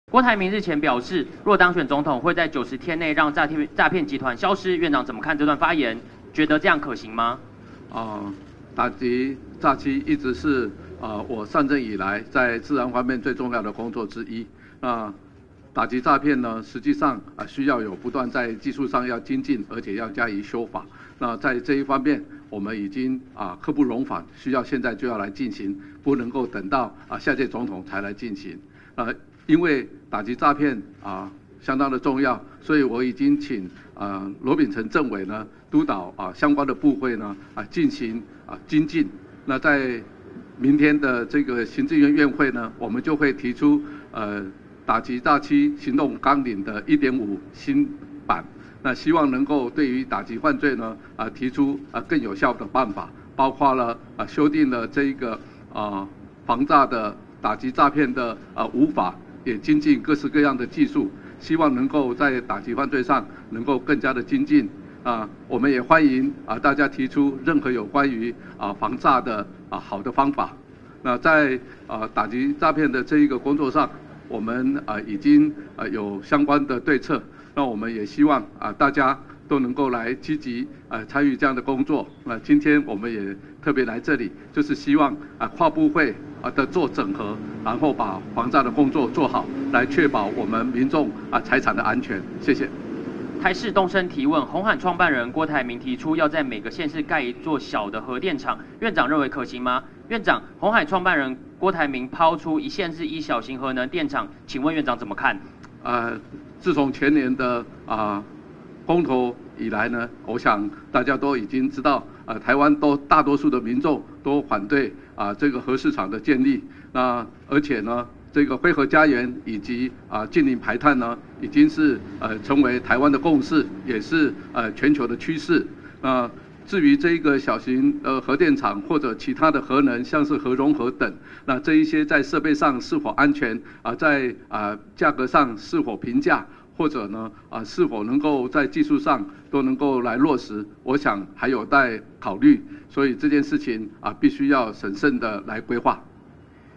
郭台銘喊「90天讓詐團消失」 陳建仁：不能等到下屆總統才進行【最新快訊】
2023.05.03【最新快訊】2024總統大選在即，積極爭取國民黨總統提名的鴻海創辦人郭台銘，日前喊話，若他當選總統，90天內會讓詐騙集團消失。法務部長蔡清祥先前對此表示，無論執行面或修法面，政府均積極偵辦打擊詐騙集團，「我們會儘快、一定會全力以赴」；而行政院長陳建仁今（3）日表示，打擊詐欺一直都是他上任以來，最重要的工作之一，「刻不容緩，現在就要進行，不能等到下屆總統才進行」。